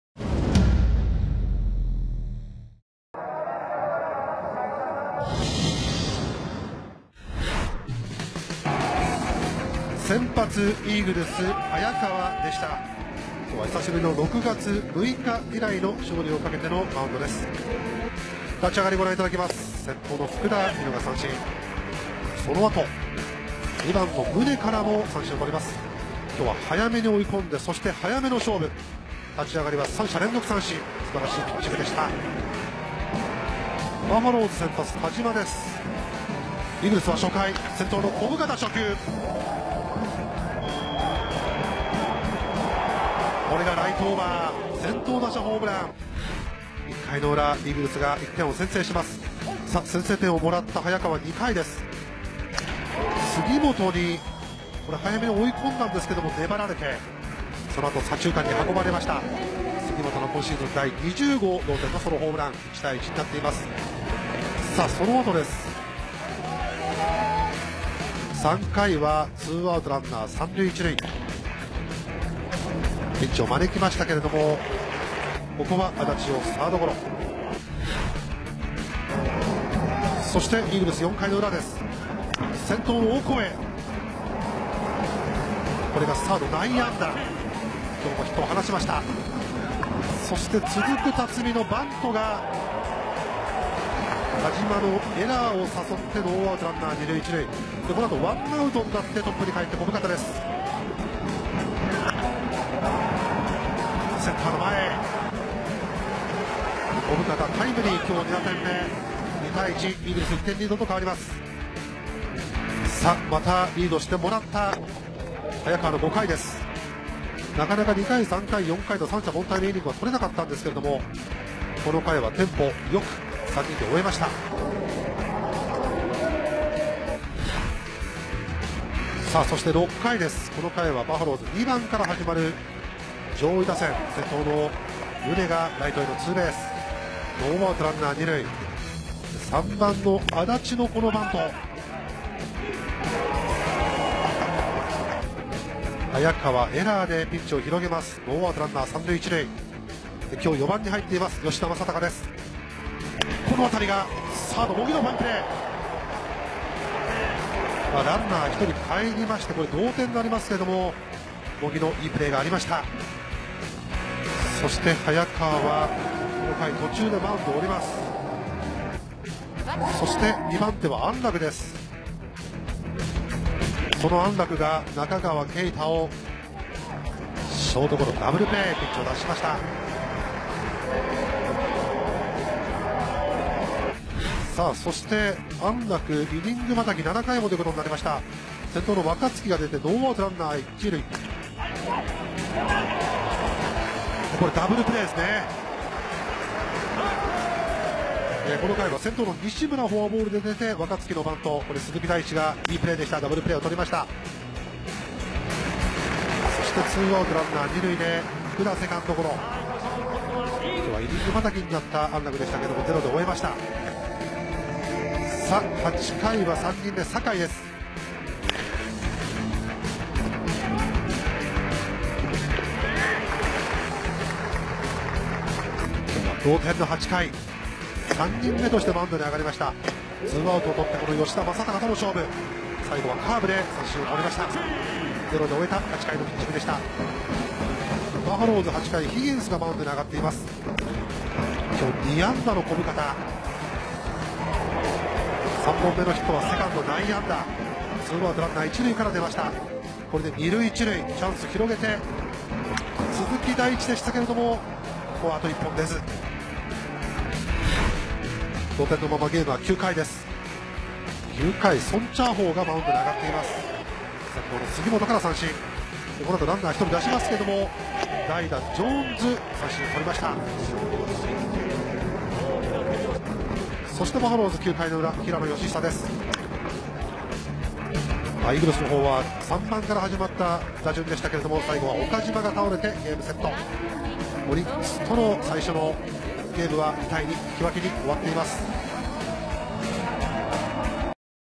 2021年8月25日 東北樂天金鷲vs歐力士猛牛 賽事亮點
2021/8/25 東北樂天金鷲 對戰 歐力士猛牛的賽事亮點影片（場地：樂天生命公園宮城球場）